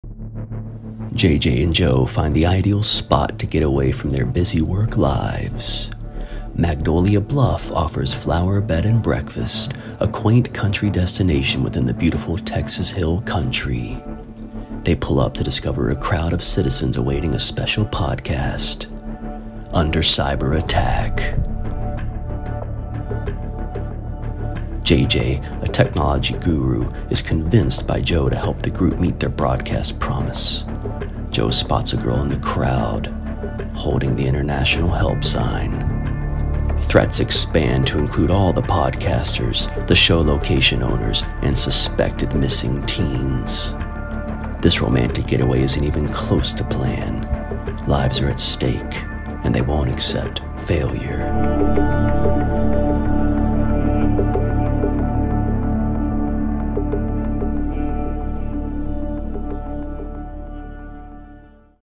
The Flower Enigma - Book Trailer
JJ and Jo find the ideal spot to get away from their busy work lives—Magnolia Bluff offers Flower Bed & Breakfast, a quaint country destination within the beautiful Texas Hill Country.
They pull up to the picturesque building only to discover a crowd of citizens anxiously awaiting a special podcast, created by a group of local women, to start. Flower B&B is the sponsor of today’s show in this multi-day series. The couple works through the human log jam. As they check in, they learn that a cyber-attack may prevent the show from airing.
JJ, a technology guru, is convinced by Jo to help the group meet their broadcast promise. While he’s working on the issue, Jo spots a girl in the crowd with fear in her eyes who flashes the international help sign. The teenager vanishes into the throng. Fearful young people signaling for help strikes too close to home for Jo.
Their romantic vacation becomes a journey from one crisis to the next as these vacationers help the evangelizing ladies even as risks are multiplying. Threats expand to include all the podcasters, the show location owners, and suspected missing teens.
This romantic getaway isn’t even close to plan. Lives are at stake, and the young couple won't accept failure.